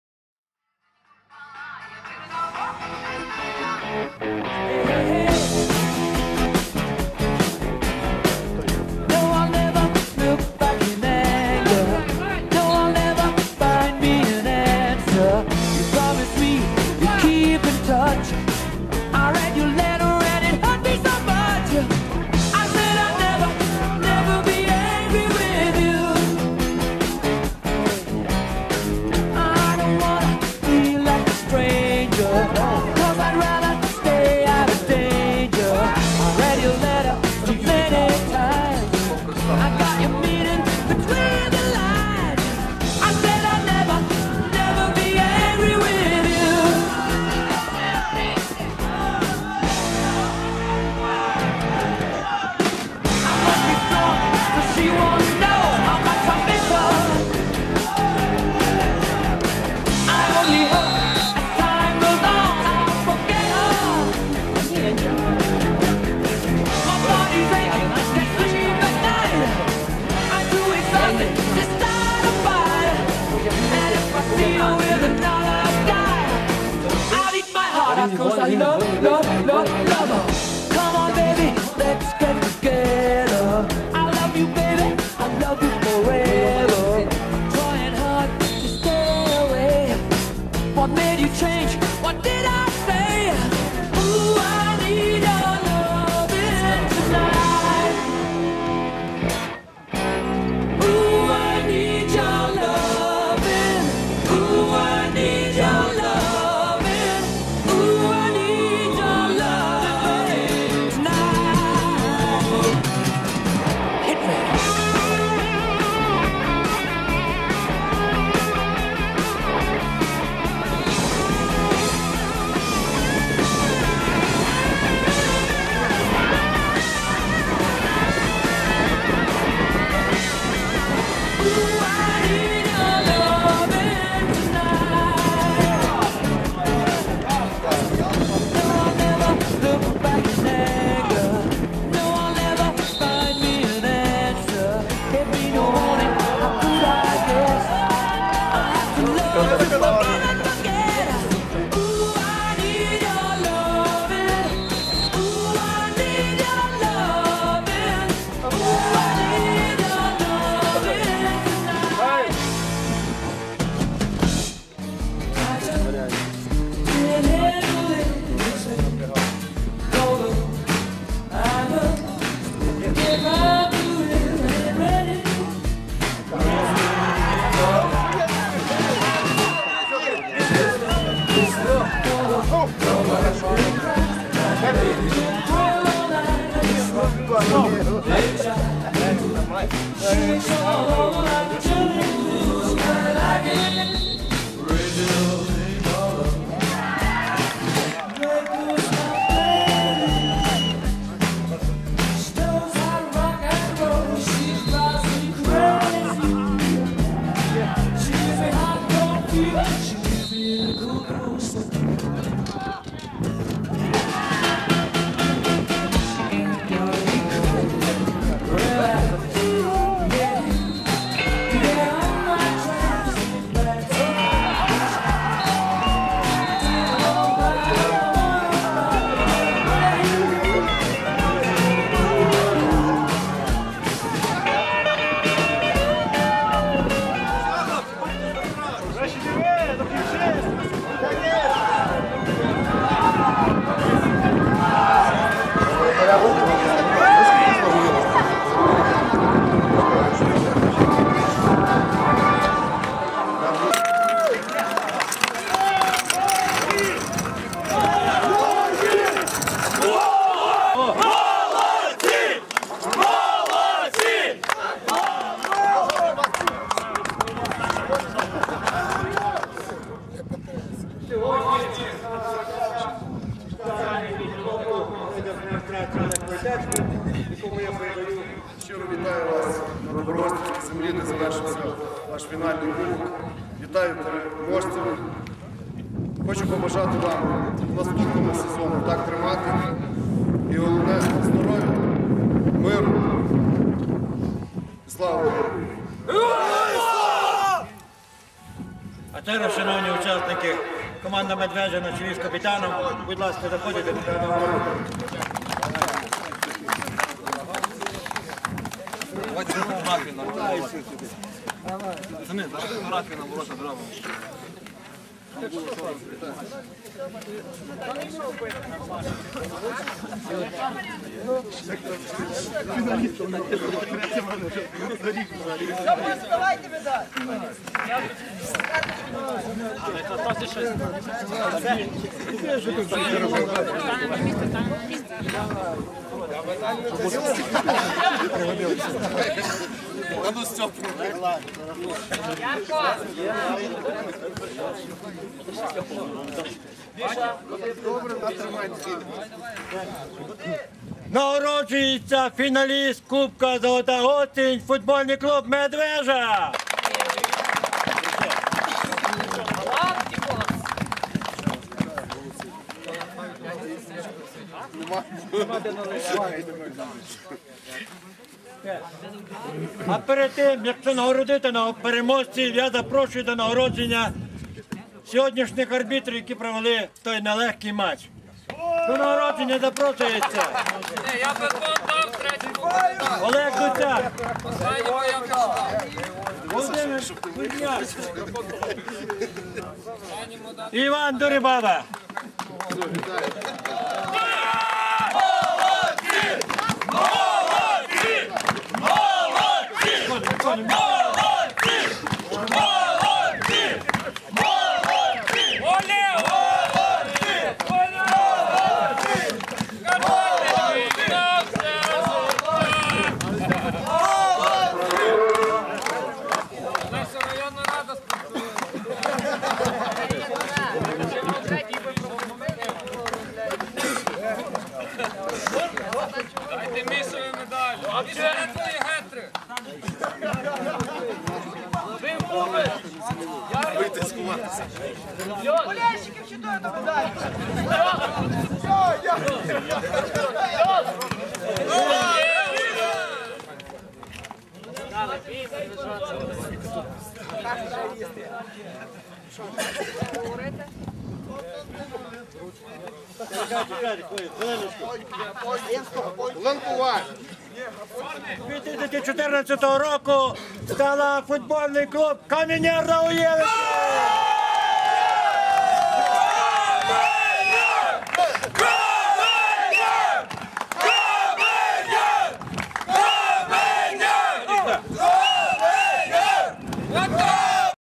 Фінал футбольного кубку "Золота осінь"
Фінал футбольного кубку "Золота осінь" відбувся на стадіоні "Карпати" в Доброгостові. Основний час зустрічі між командами "Каменяр" (Нагуєвичі) та ФК "Медвежа" завершився внічию 2:2. В серії післяматчевих пенельті з рахунком 6:5 перемогу святкували футболісти з Нагуєвич.